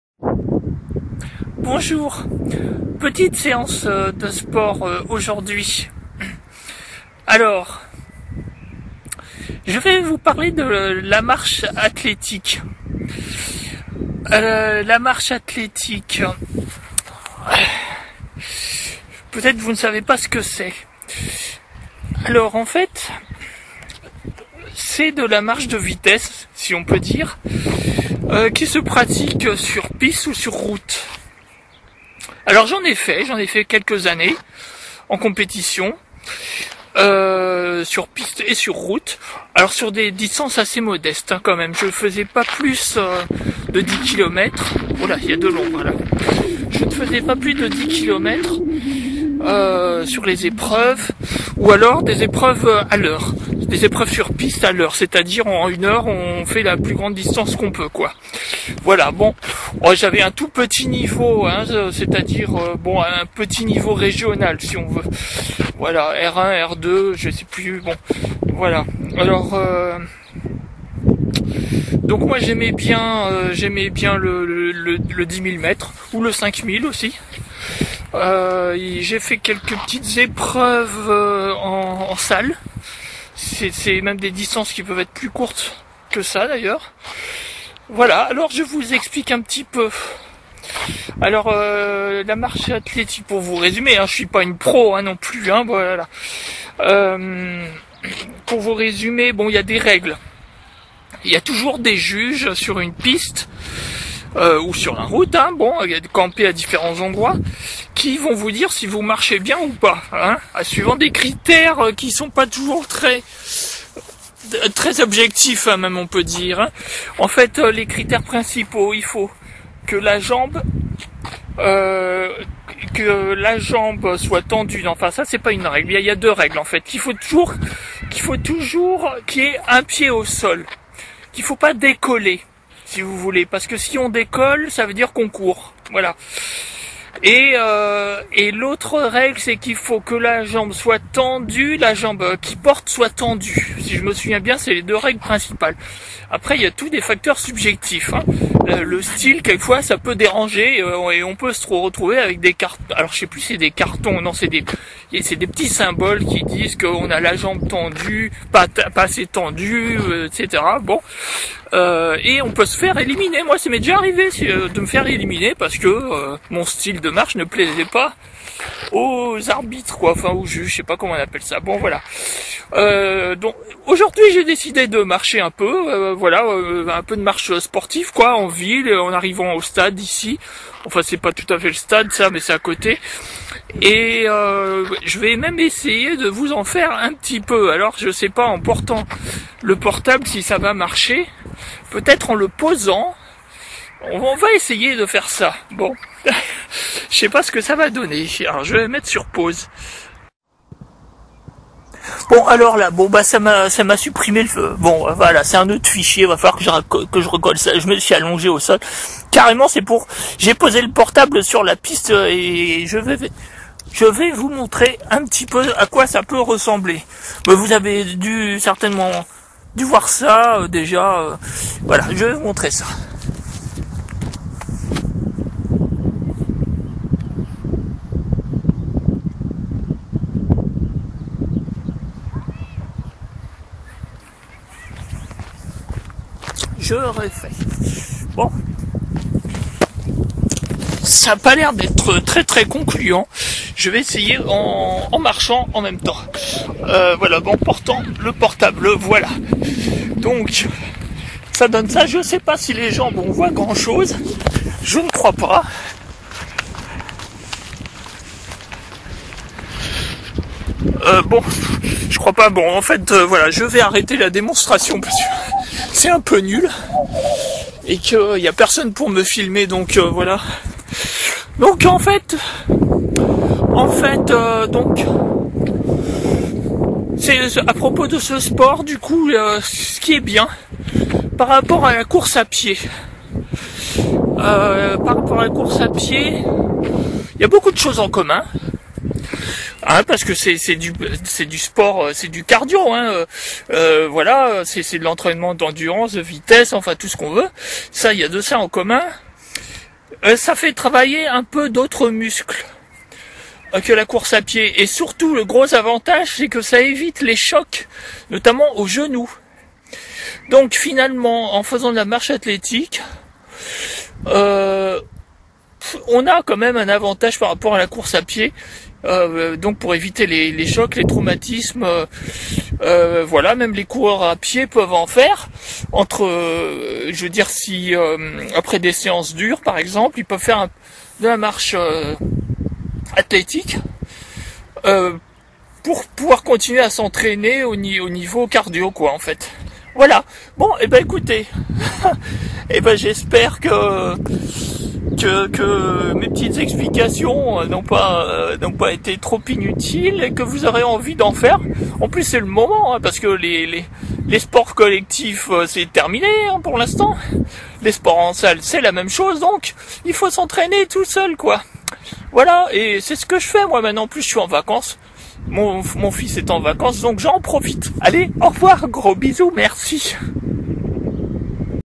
la marche athlétique... présentation de cette pratique sportive
la marche atjlétique est un sport peu connu, j'en ai pratiqué pendant plusieurs années...
merci à Jacqueline Bonnet, multiple championne de France qui m'a mise sur les rails..
Merci à ce marcheur russe (dont j'ai oublié le nom) que j'ai accompagné au Paris Colmar et qui m'a fait découvrir cette prestigieuse épreuve